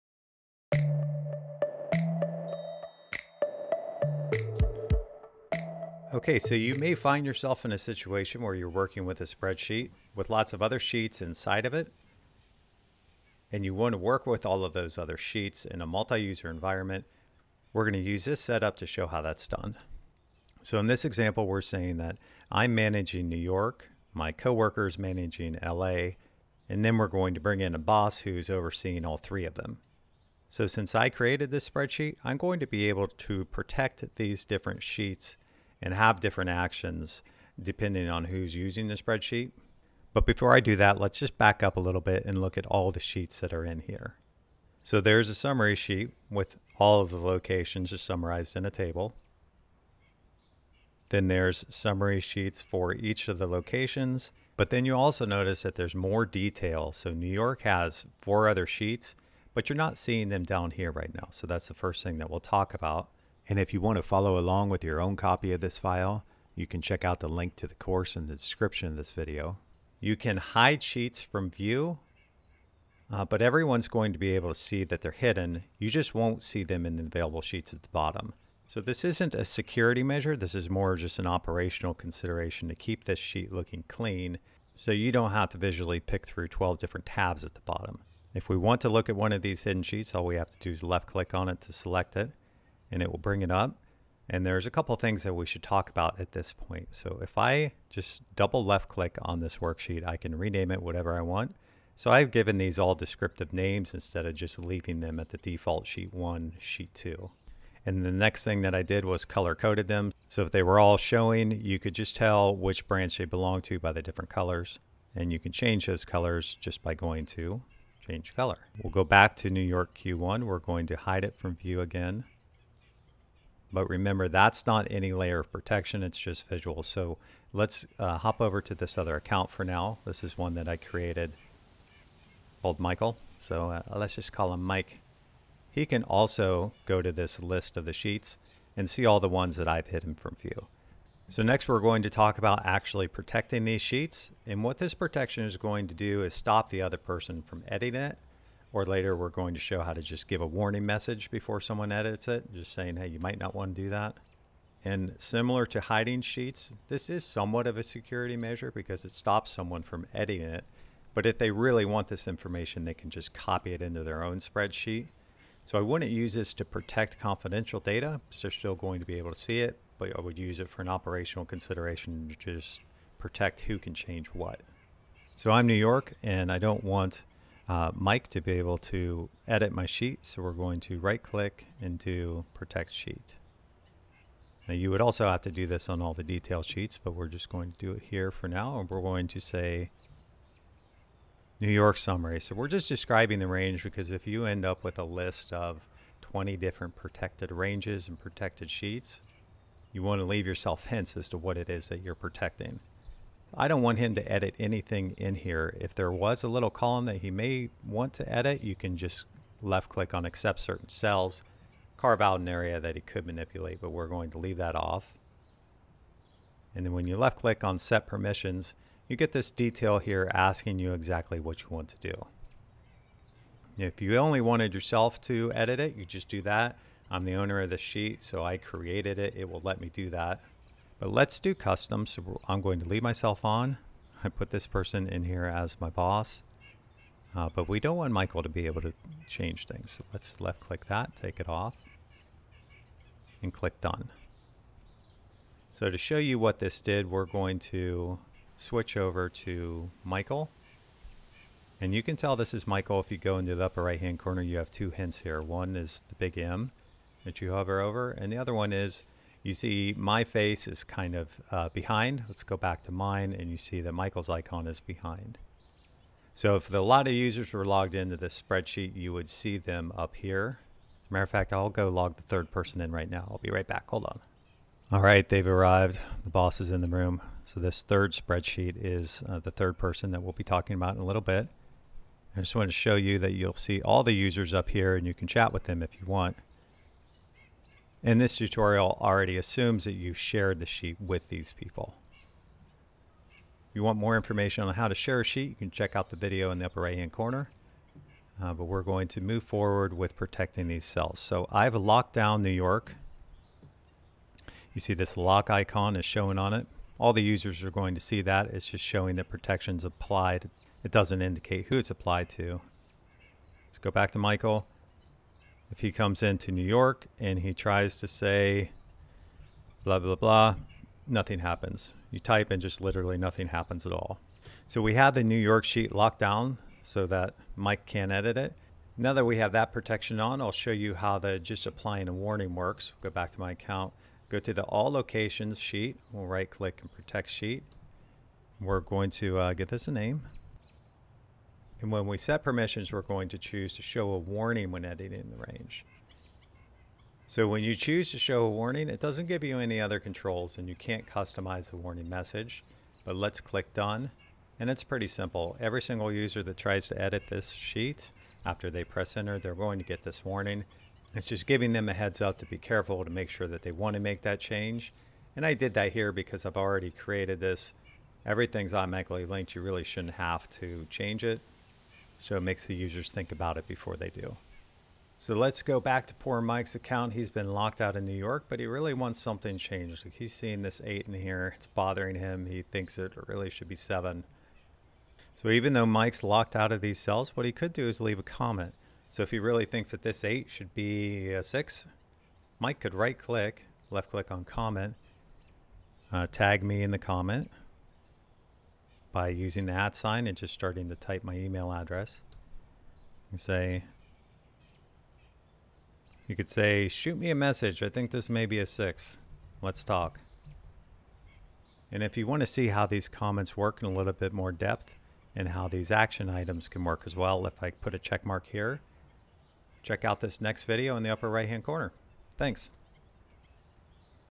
Google Sheets - Protect, Customize, and Hide Sheets | Multiple Users
Learn how to organize the sheets in a large Google Sheets file where multiple users need different access levels to different sheets. Learn to rename, color, hide and protect different sheets.

🕒 Timestamps:
0:00 Intro
0:55 Hide sheets
1:38 Rename sheets
1:48 Color code
2:22 Protect sheets
5:29 Give a warning